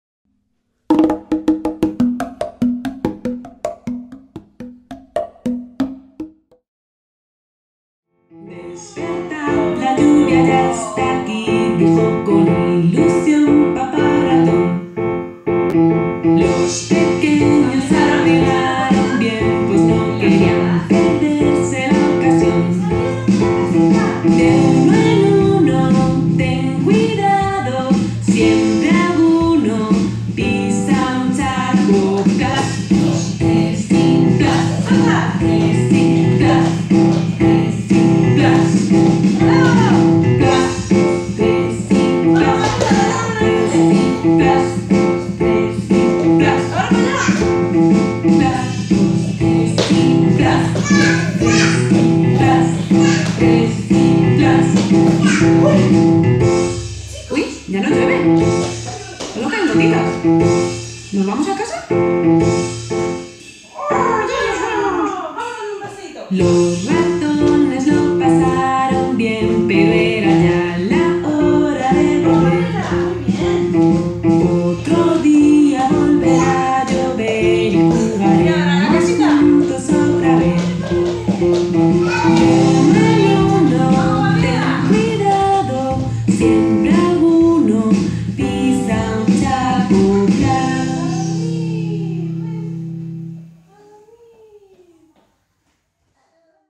Los Ratones y la lluvia: música y movimiento
OBJETIVOS:

- Incorporar rutinas, como abrigarse antes de salir.
- Asimilación y normalización del fin de una situación de juego, una vez que deja de ser un momento propicio.
- Desarrollo cognitivo con el trabajo del ritmo.
- Mejora de la motricidad gruesa, la coordinación y la ubicación espacial.
- Favorecimiento de la espera de turnos y la socialización.

Pincha en el siguiente link y abre la nota con tips para usarla en tus sesiones: